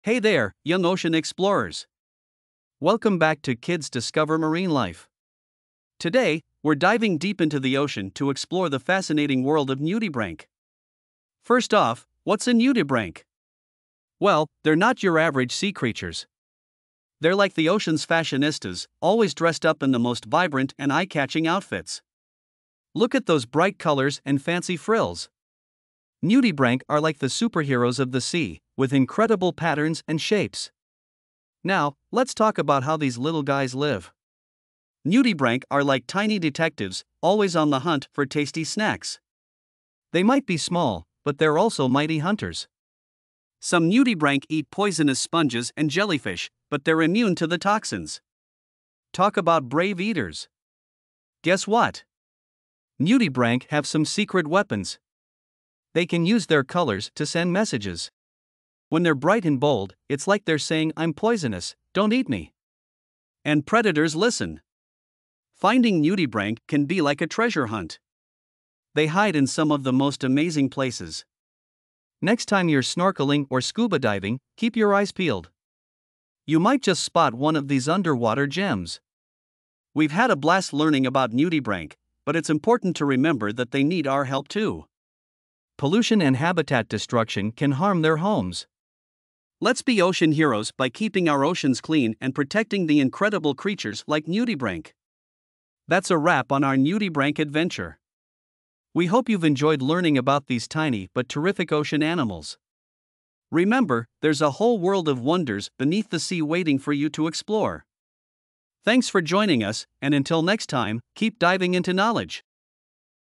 Nudibranch Adventures The Colorful World Beneath
🌊 Dive into the mesmerizing world of Nudibranchs! 🌈 Join us on an underwater adventure as we uncover the secrets of these colorful sea slugs. Discover their stunning outfits, their unique behaviors, and the important role they play in the ocean's ecosystem. Get ready for an exploration that will leave you in awe of nature's creativity!